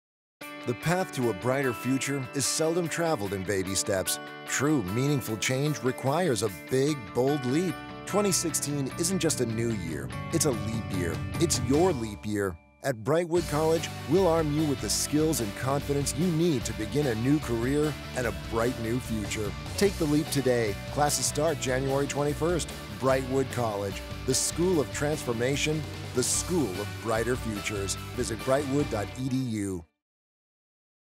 Giant Leap New Year: Brightwood College
Meaningful change requires a big, bold leap. Take the leap toward a new career and a brighter future with an education from Brightwood College. Classes start January 21.